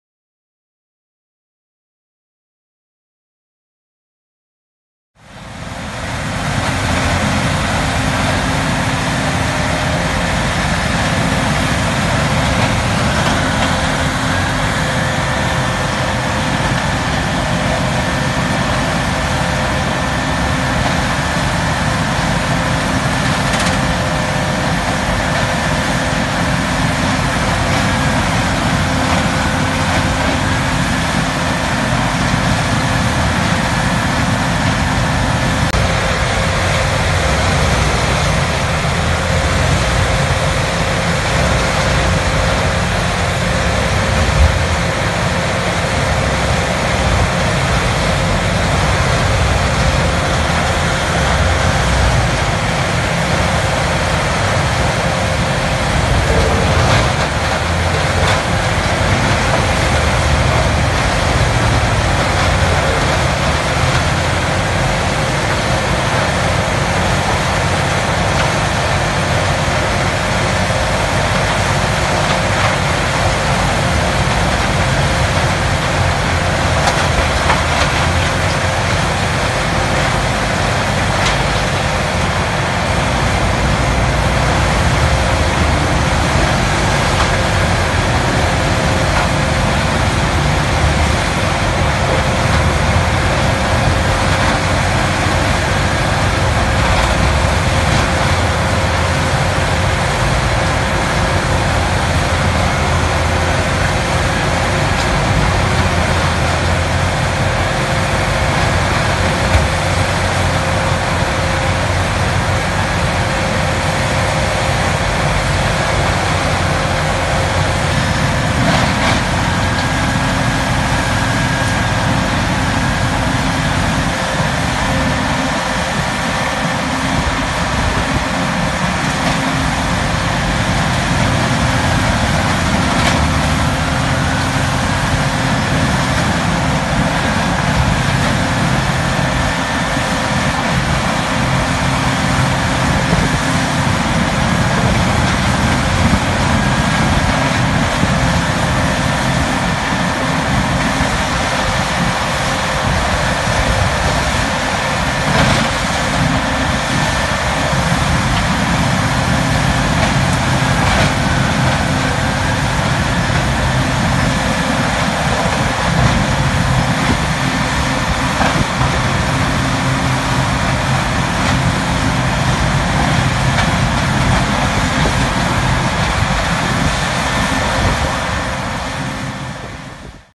Morbark 6400XT Wood Hog krossar ris och kompost
Morbark Wood Hog 6400XT krossar ris oh kompost, CAT C27 dieselmotor 1050 hk. Enorm kapacitet!